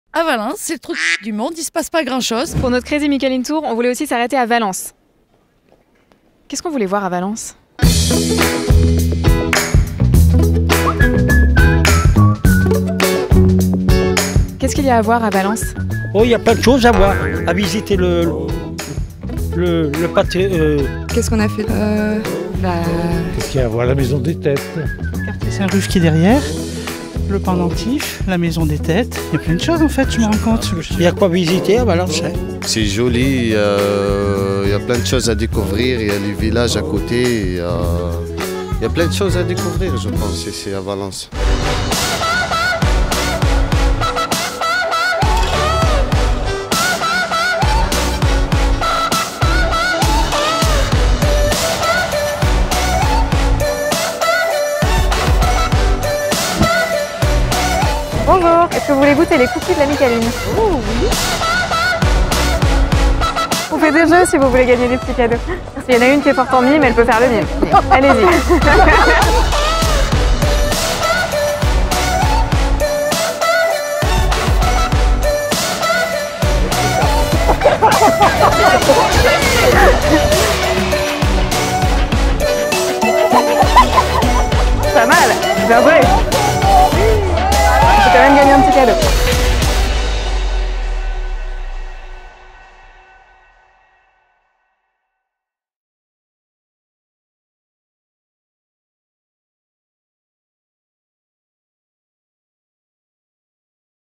Le CrazyMieCâlineTour à Valence
En 2019, La Mie Câline réédite son CrazyMieCalineTour à travers la France ! Le périple a commencé à Avignon puis Valence et s'est terminé à Annecy.
Bilan de cette édition : 2243 km parcourus, près de 1 000 mini-cookies distribués, des dizaines de mîmes improbables, 1 trousse de toilette oubliée à l’hôtel… et beaucoup trop de sourires pour que l’on ait pu les compter !

On est plutôt du style à réserver des petites surprises sympas à ceux qui partagent notre bonne humeur, alors suivez-vous ;-)

Réalisation : by the way Prod